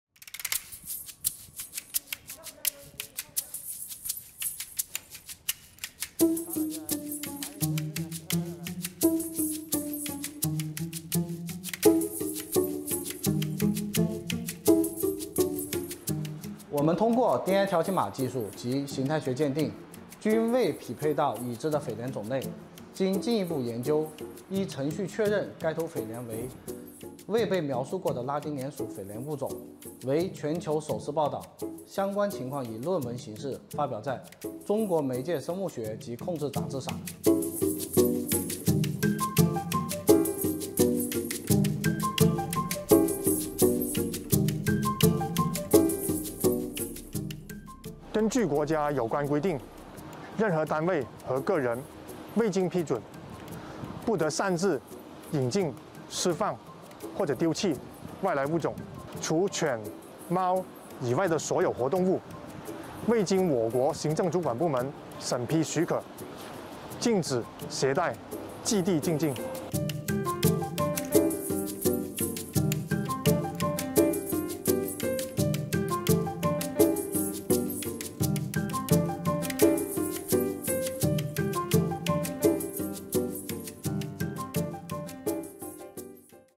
Chinese customs officers accidentally discover new cockroach species in surprise haul
Customs officials at a port in Zhuhai in the southern Chinese province of Guangdong made a surprise discovery of a new species of cockroach while inspecting a shipment of logs infested with the insects on November 10, 2022. Experts at a laboratory at Gongbei Port later determined that the cockroaches found were a previously unidentified species from the Periplaneta latina genus.